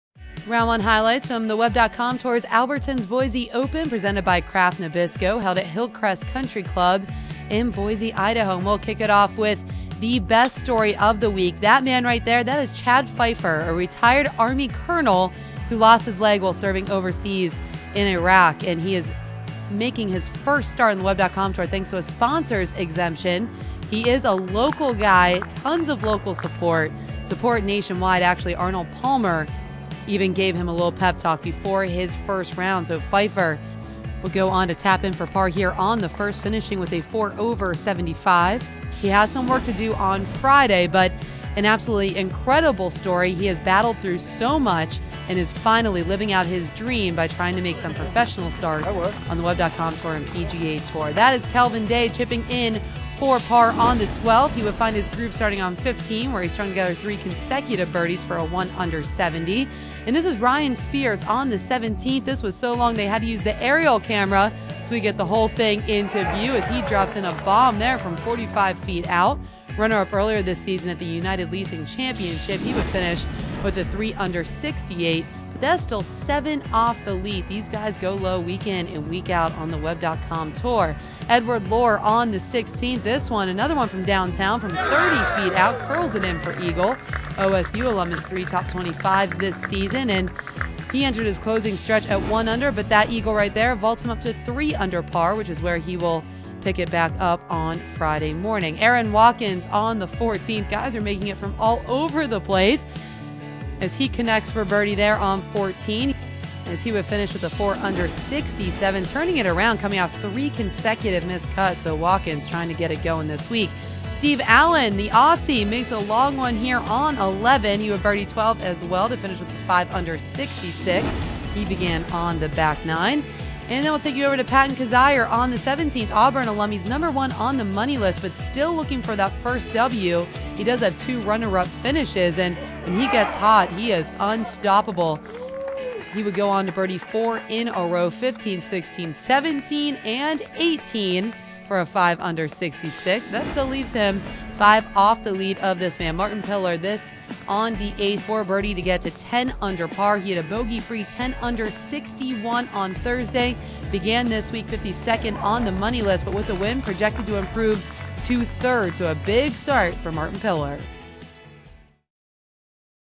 Highlights | Martin Piller boasts a 61 to lead by three at the Albertsons Boise Open
In the opening round of the 2015 Albertsons Boise Open presented by Kraft Nabisco, Martin Piller had a stellar day firing off 10 birdies to take a three shot lead over Erik Barnes and Zach Fischer.